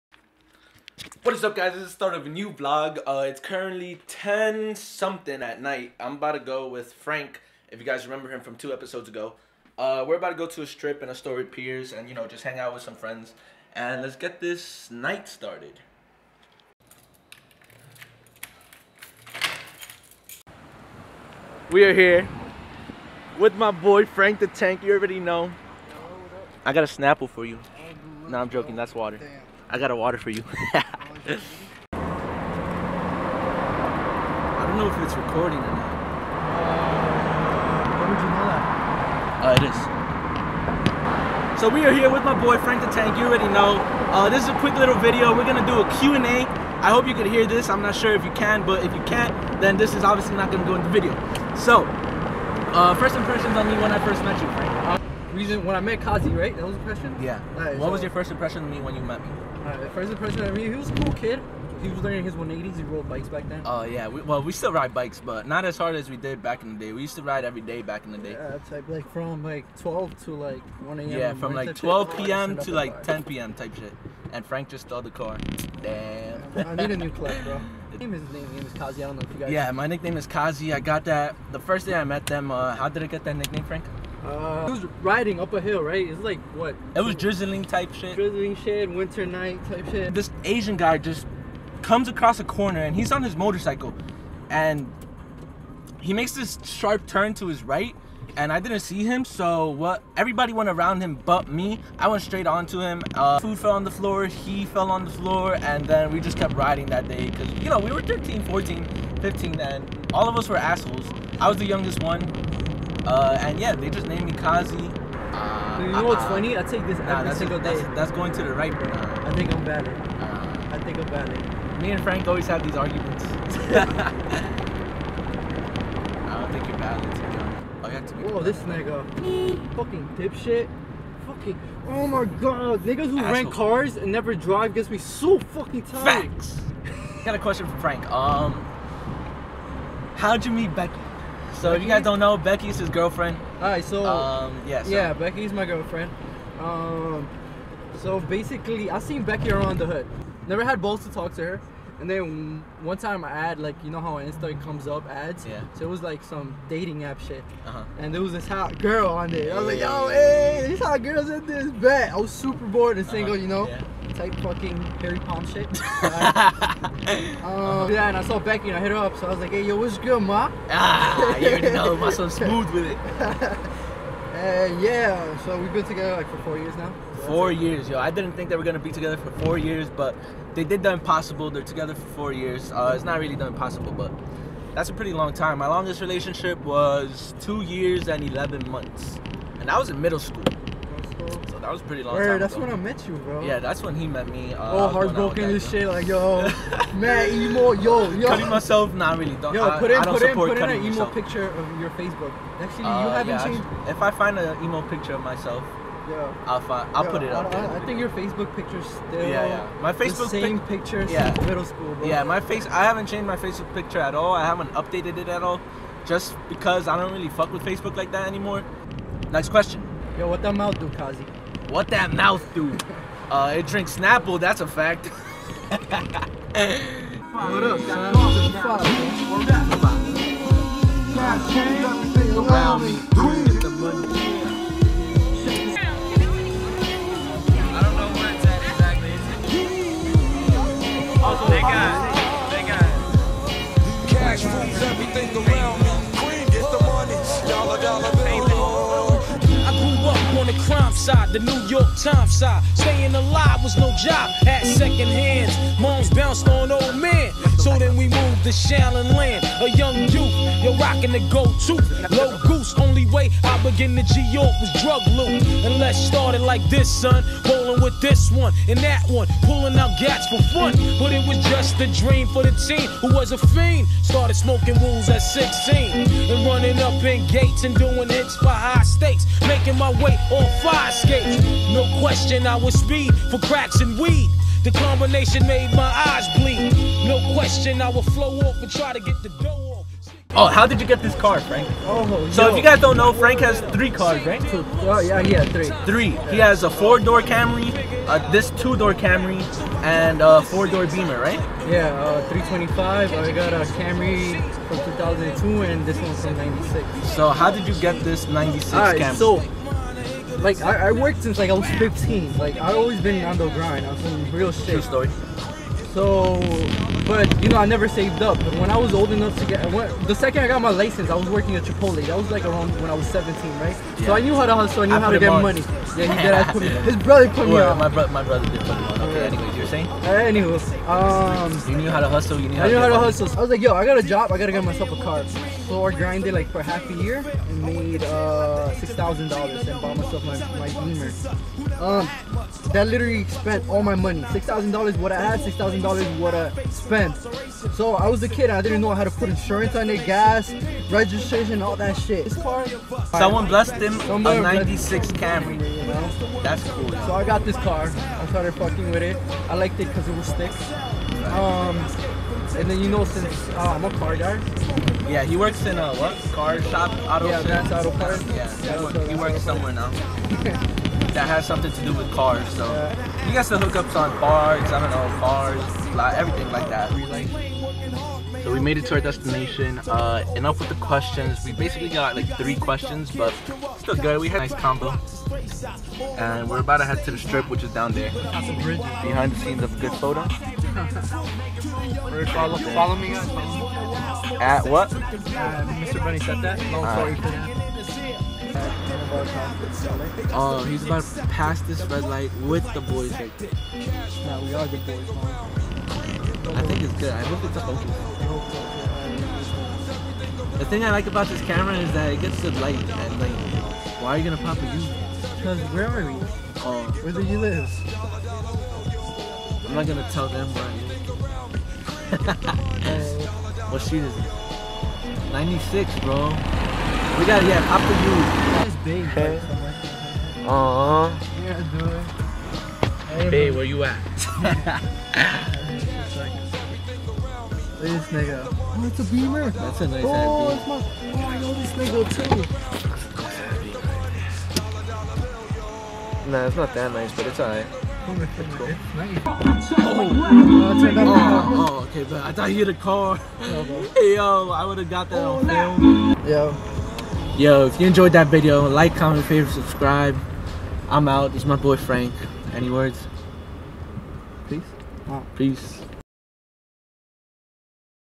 Q&A WITH FRANK THE TANK !
me and frank went to meet up with the homies at astoria strip and we decided to do a quick q&a. ENJOY !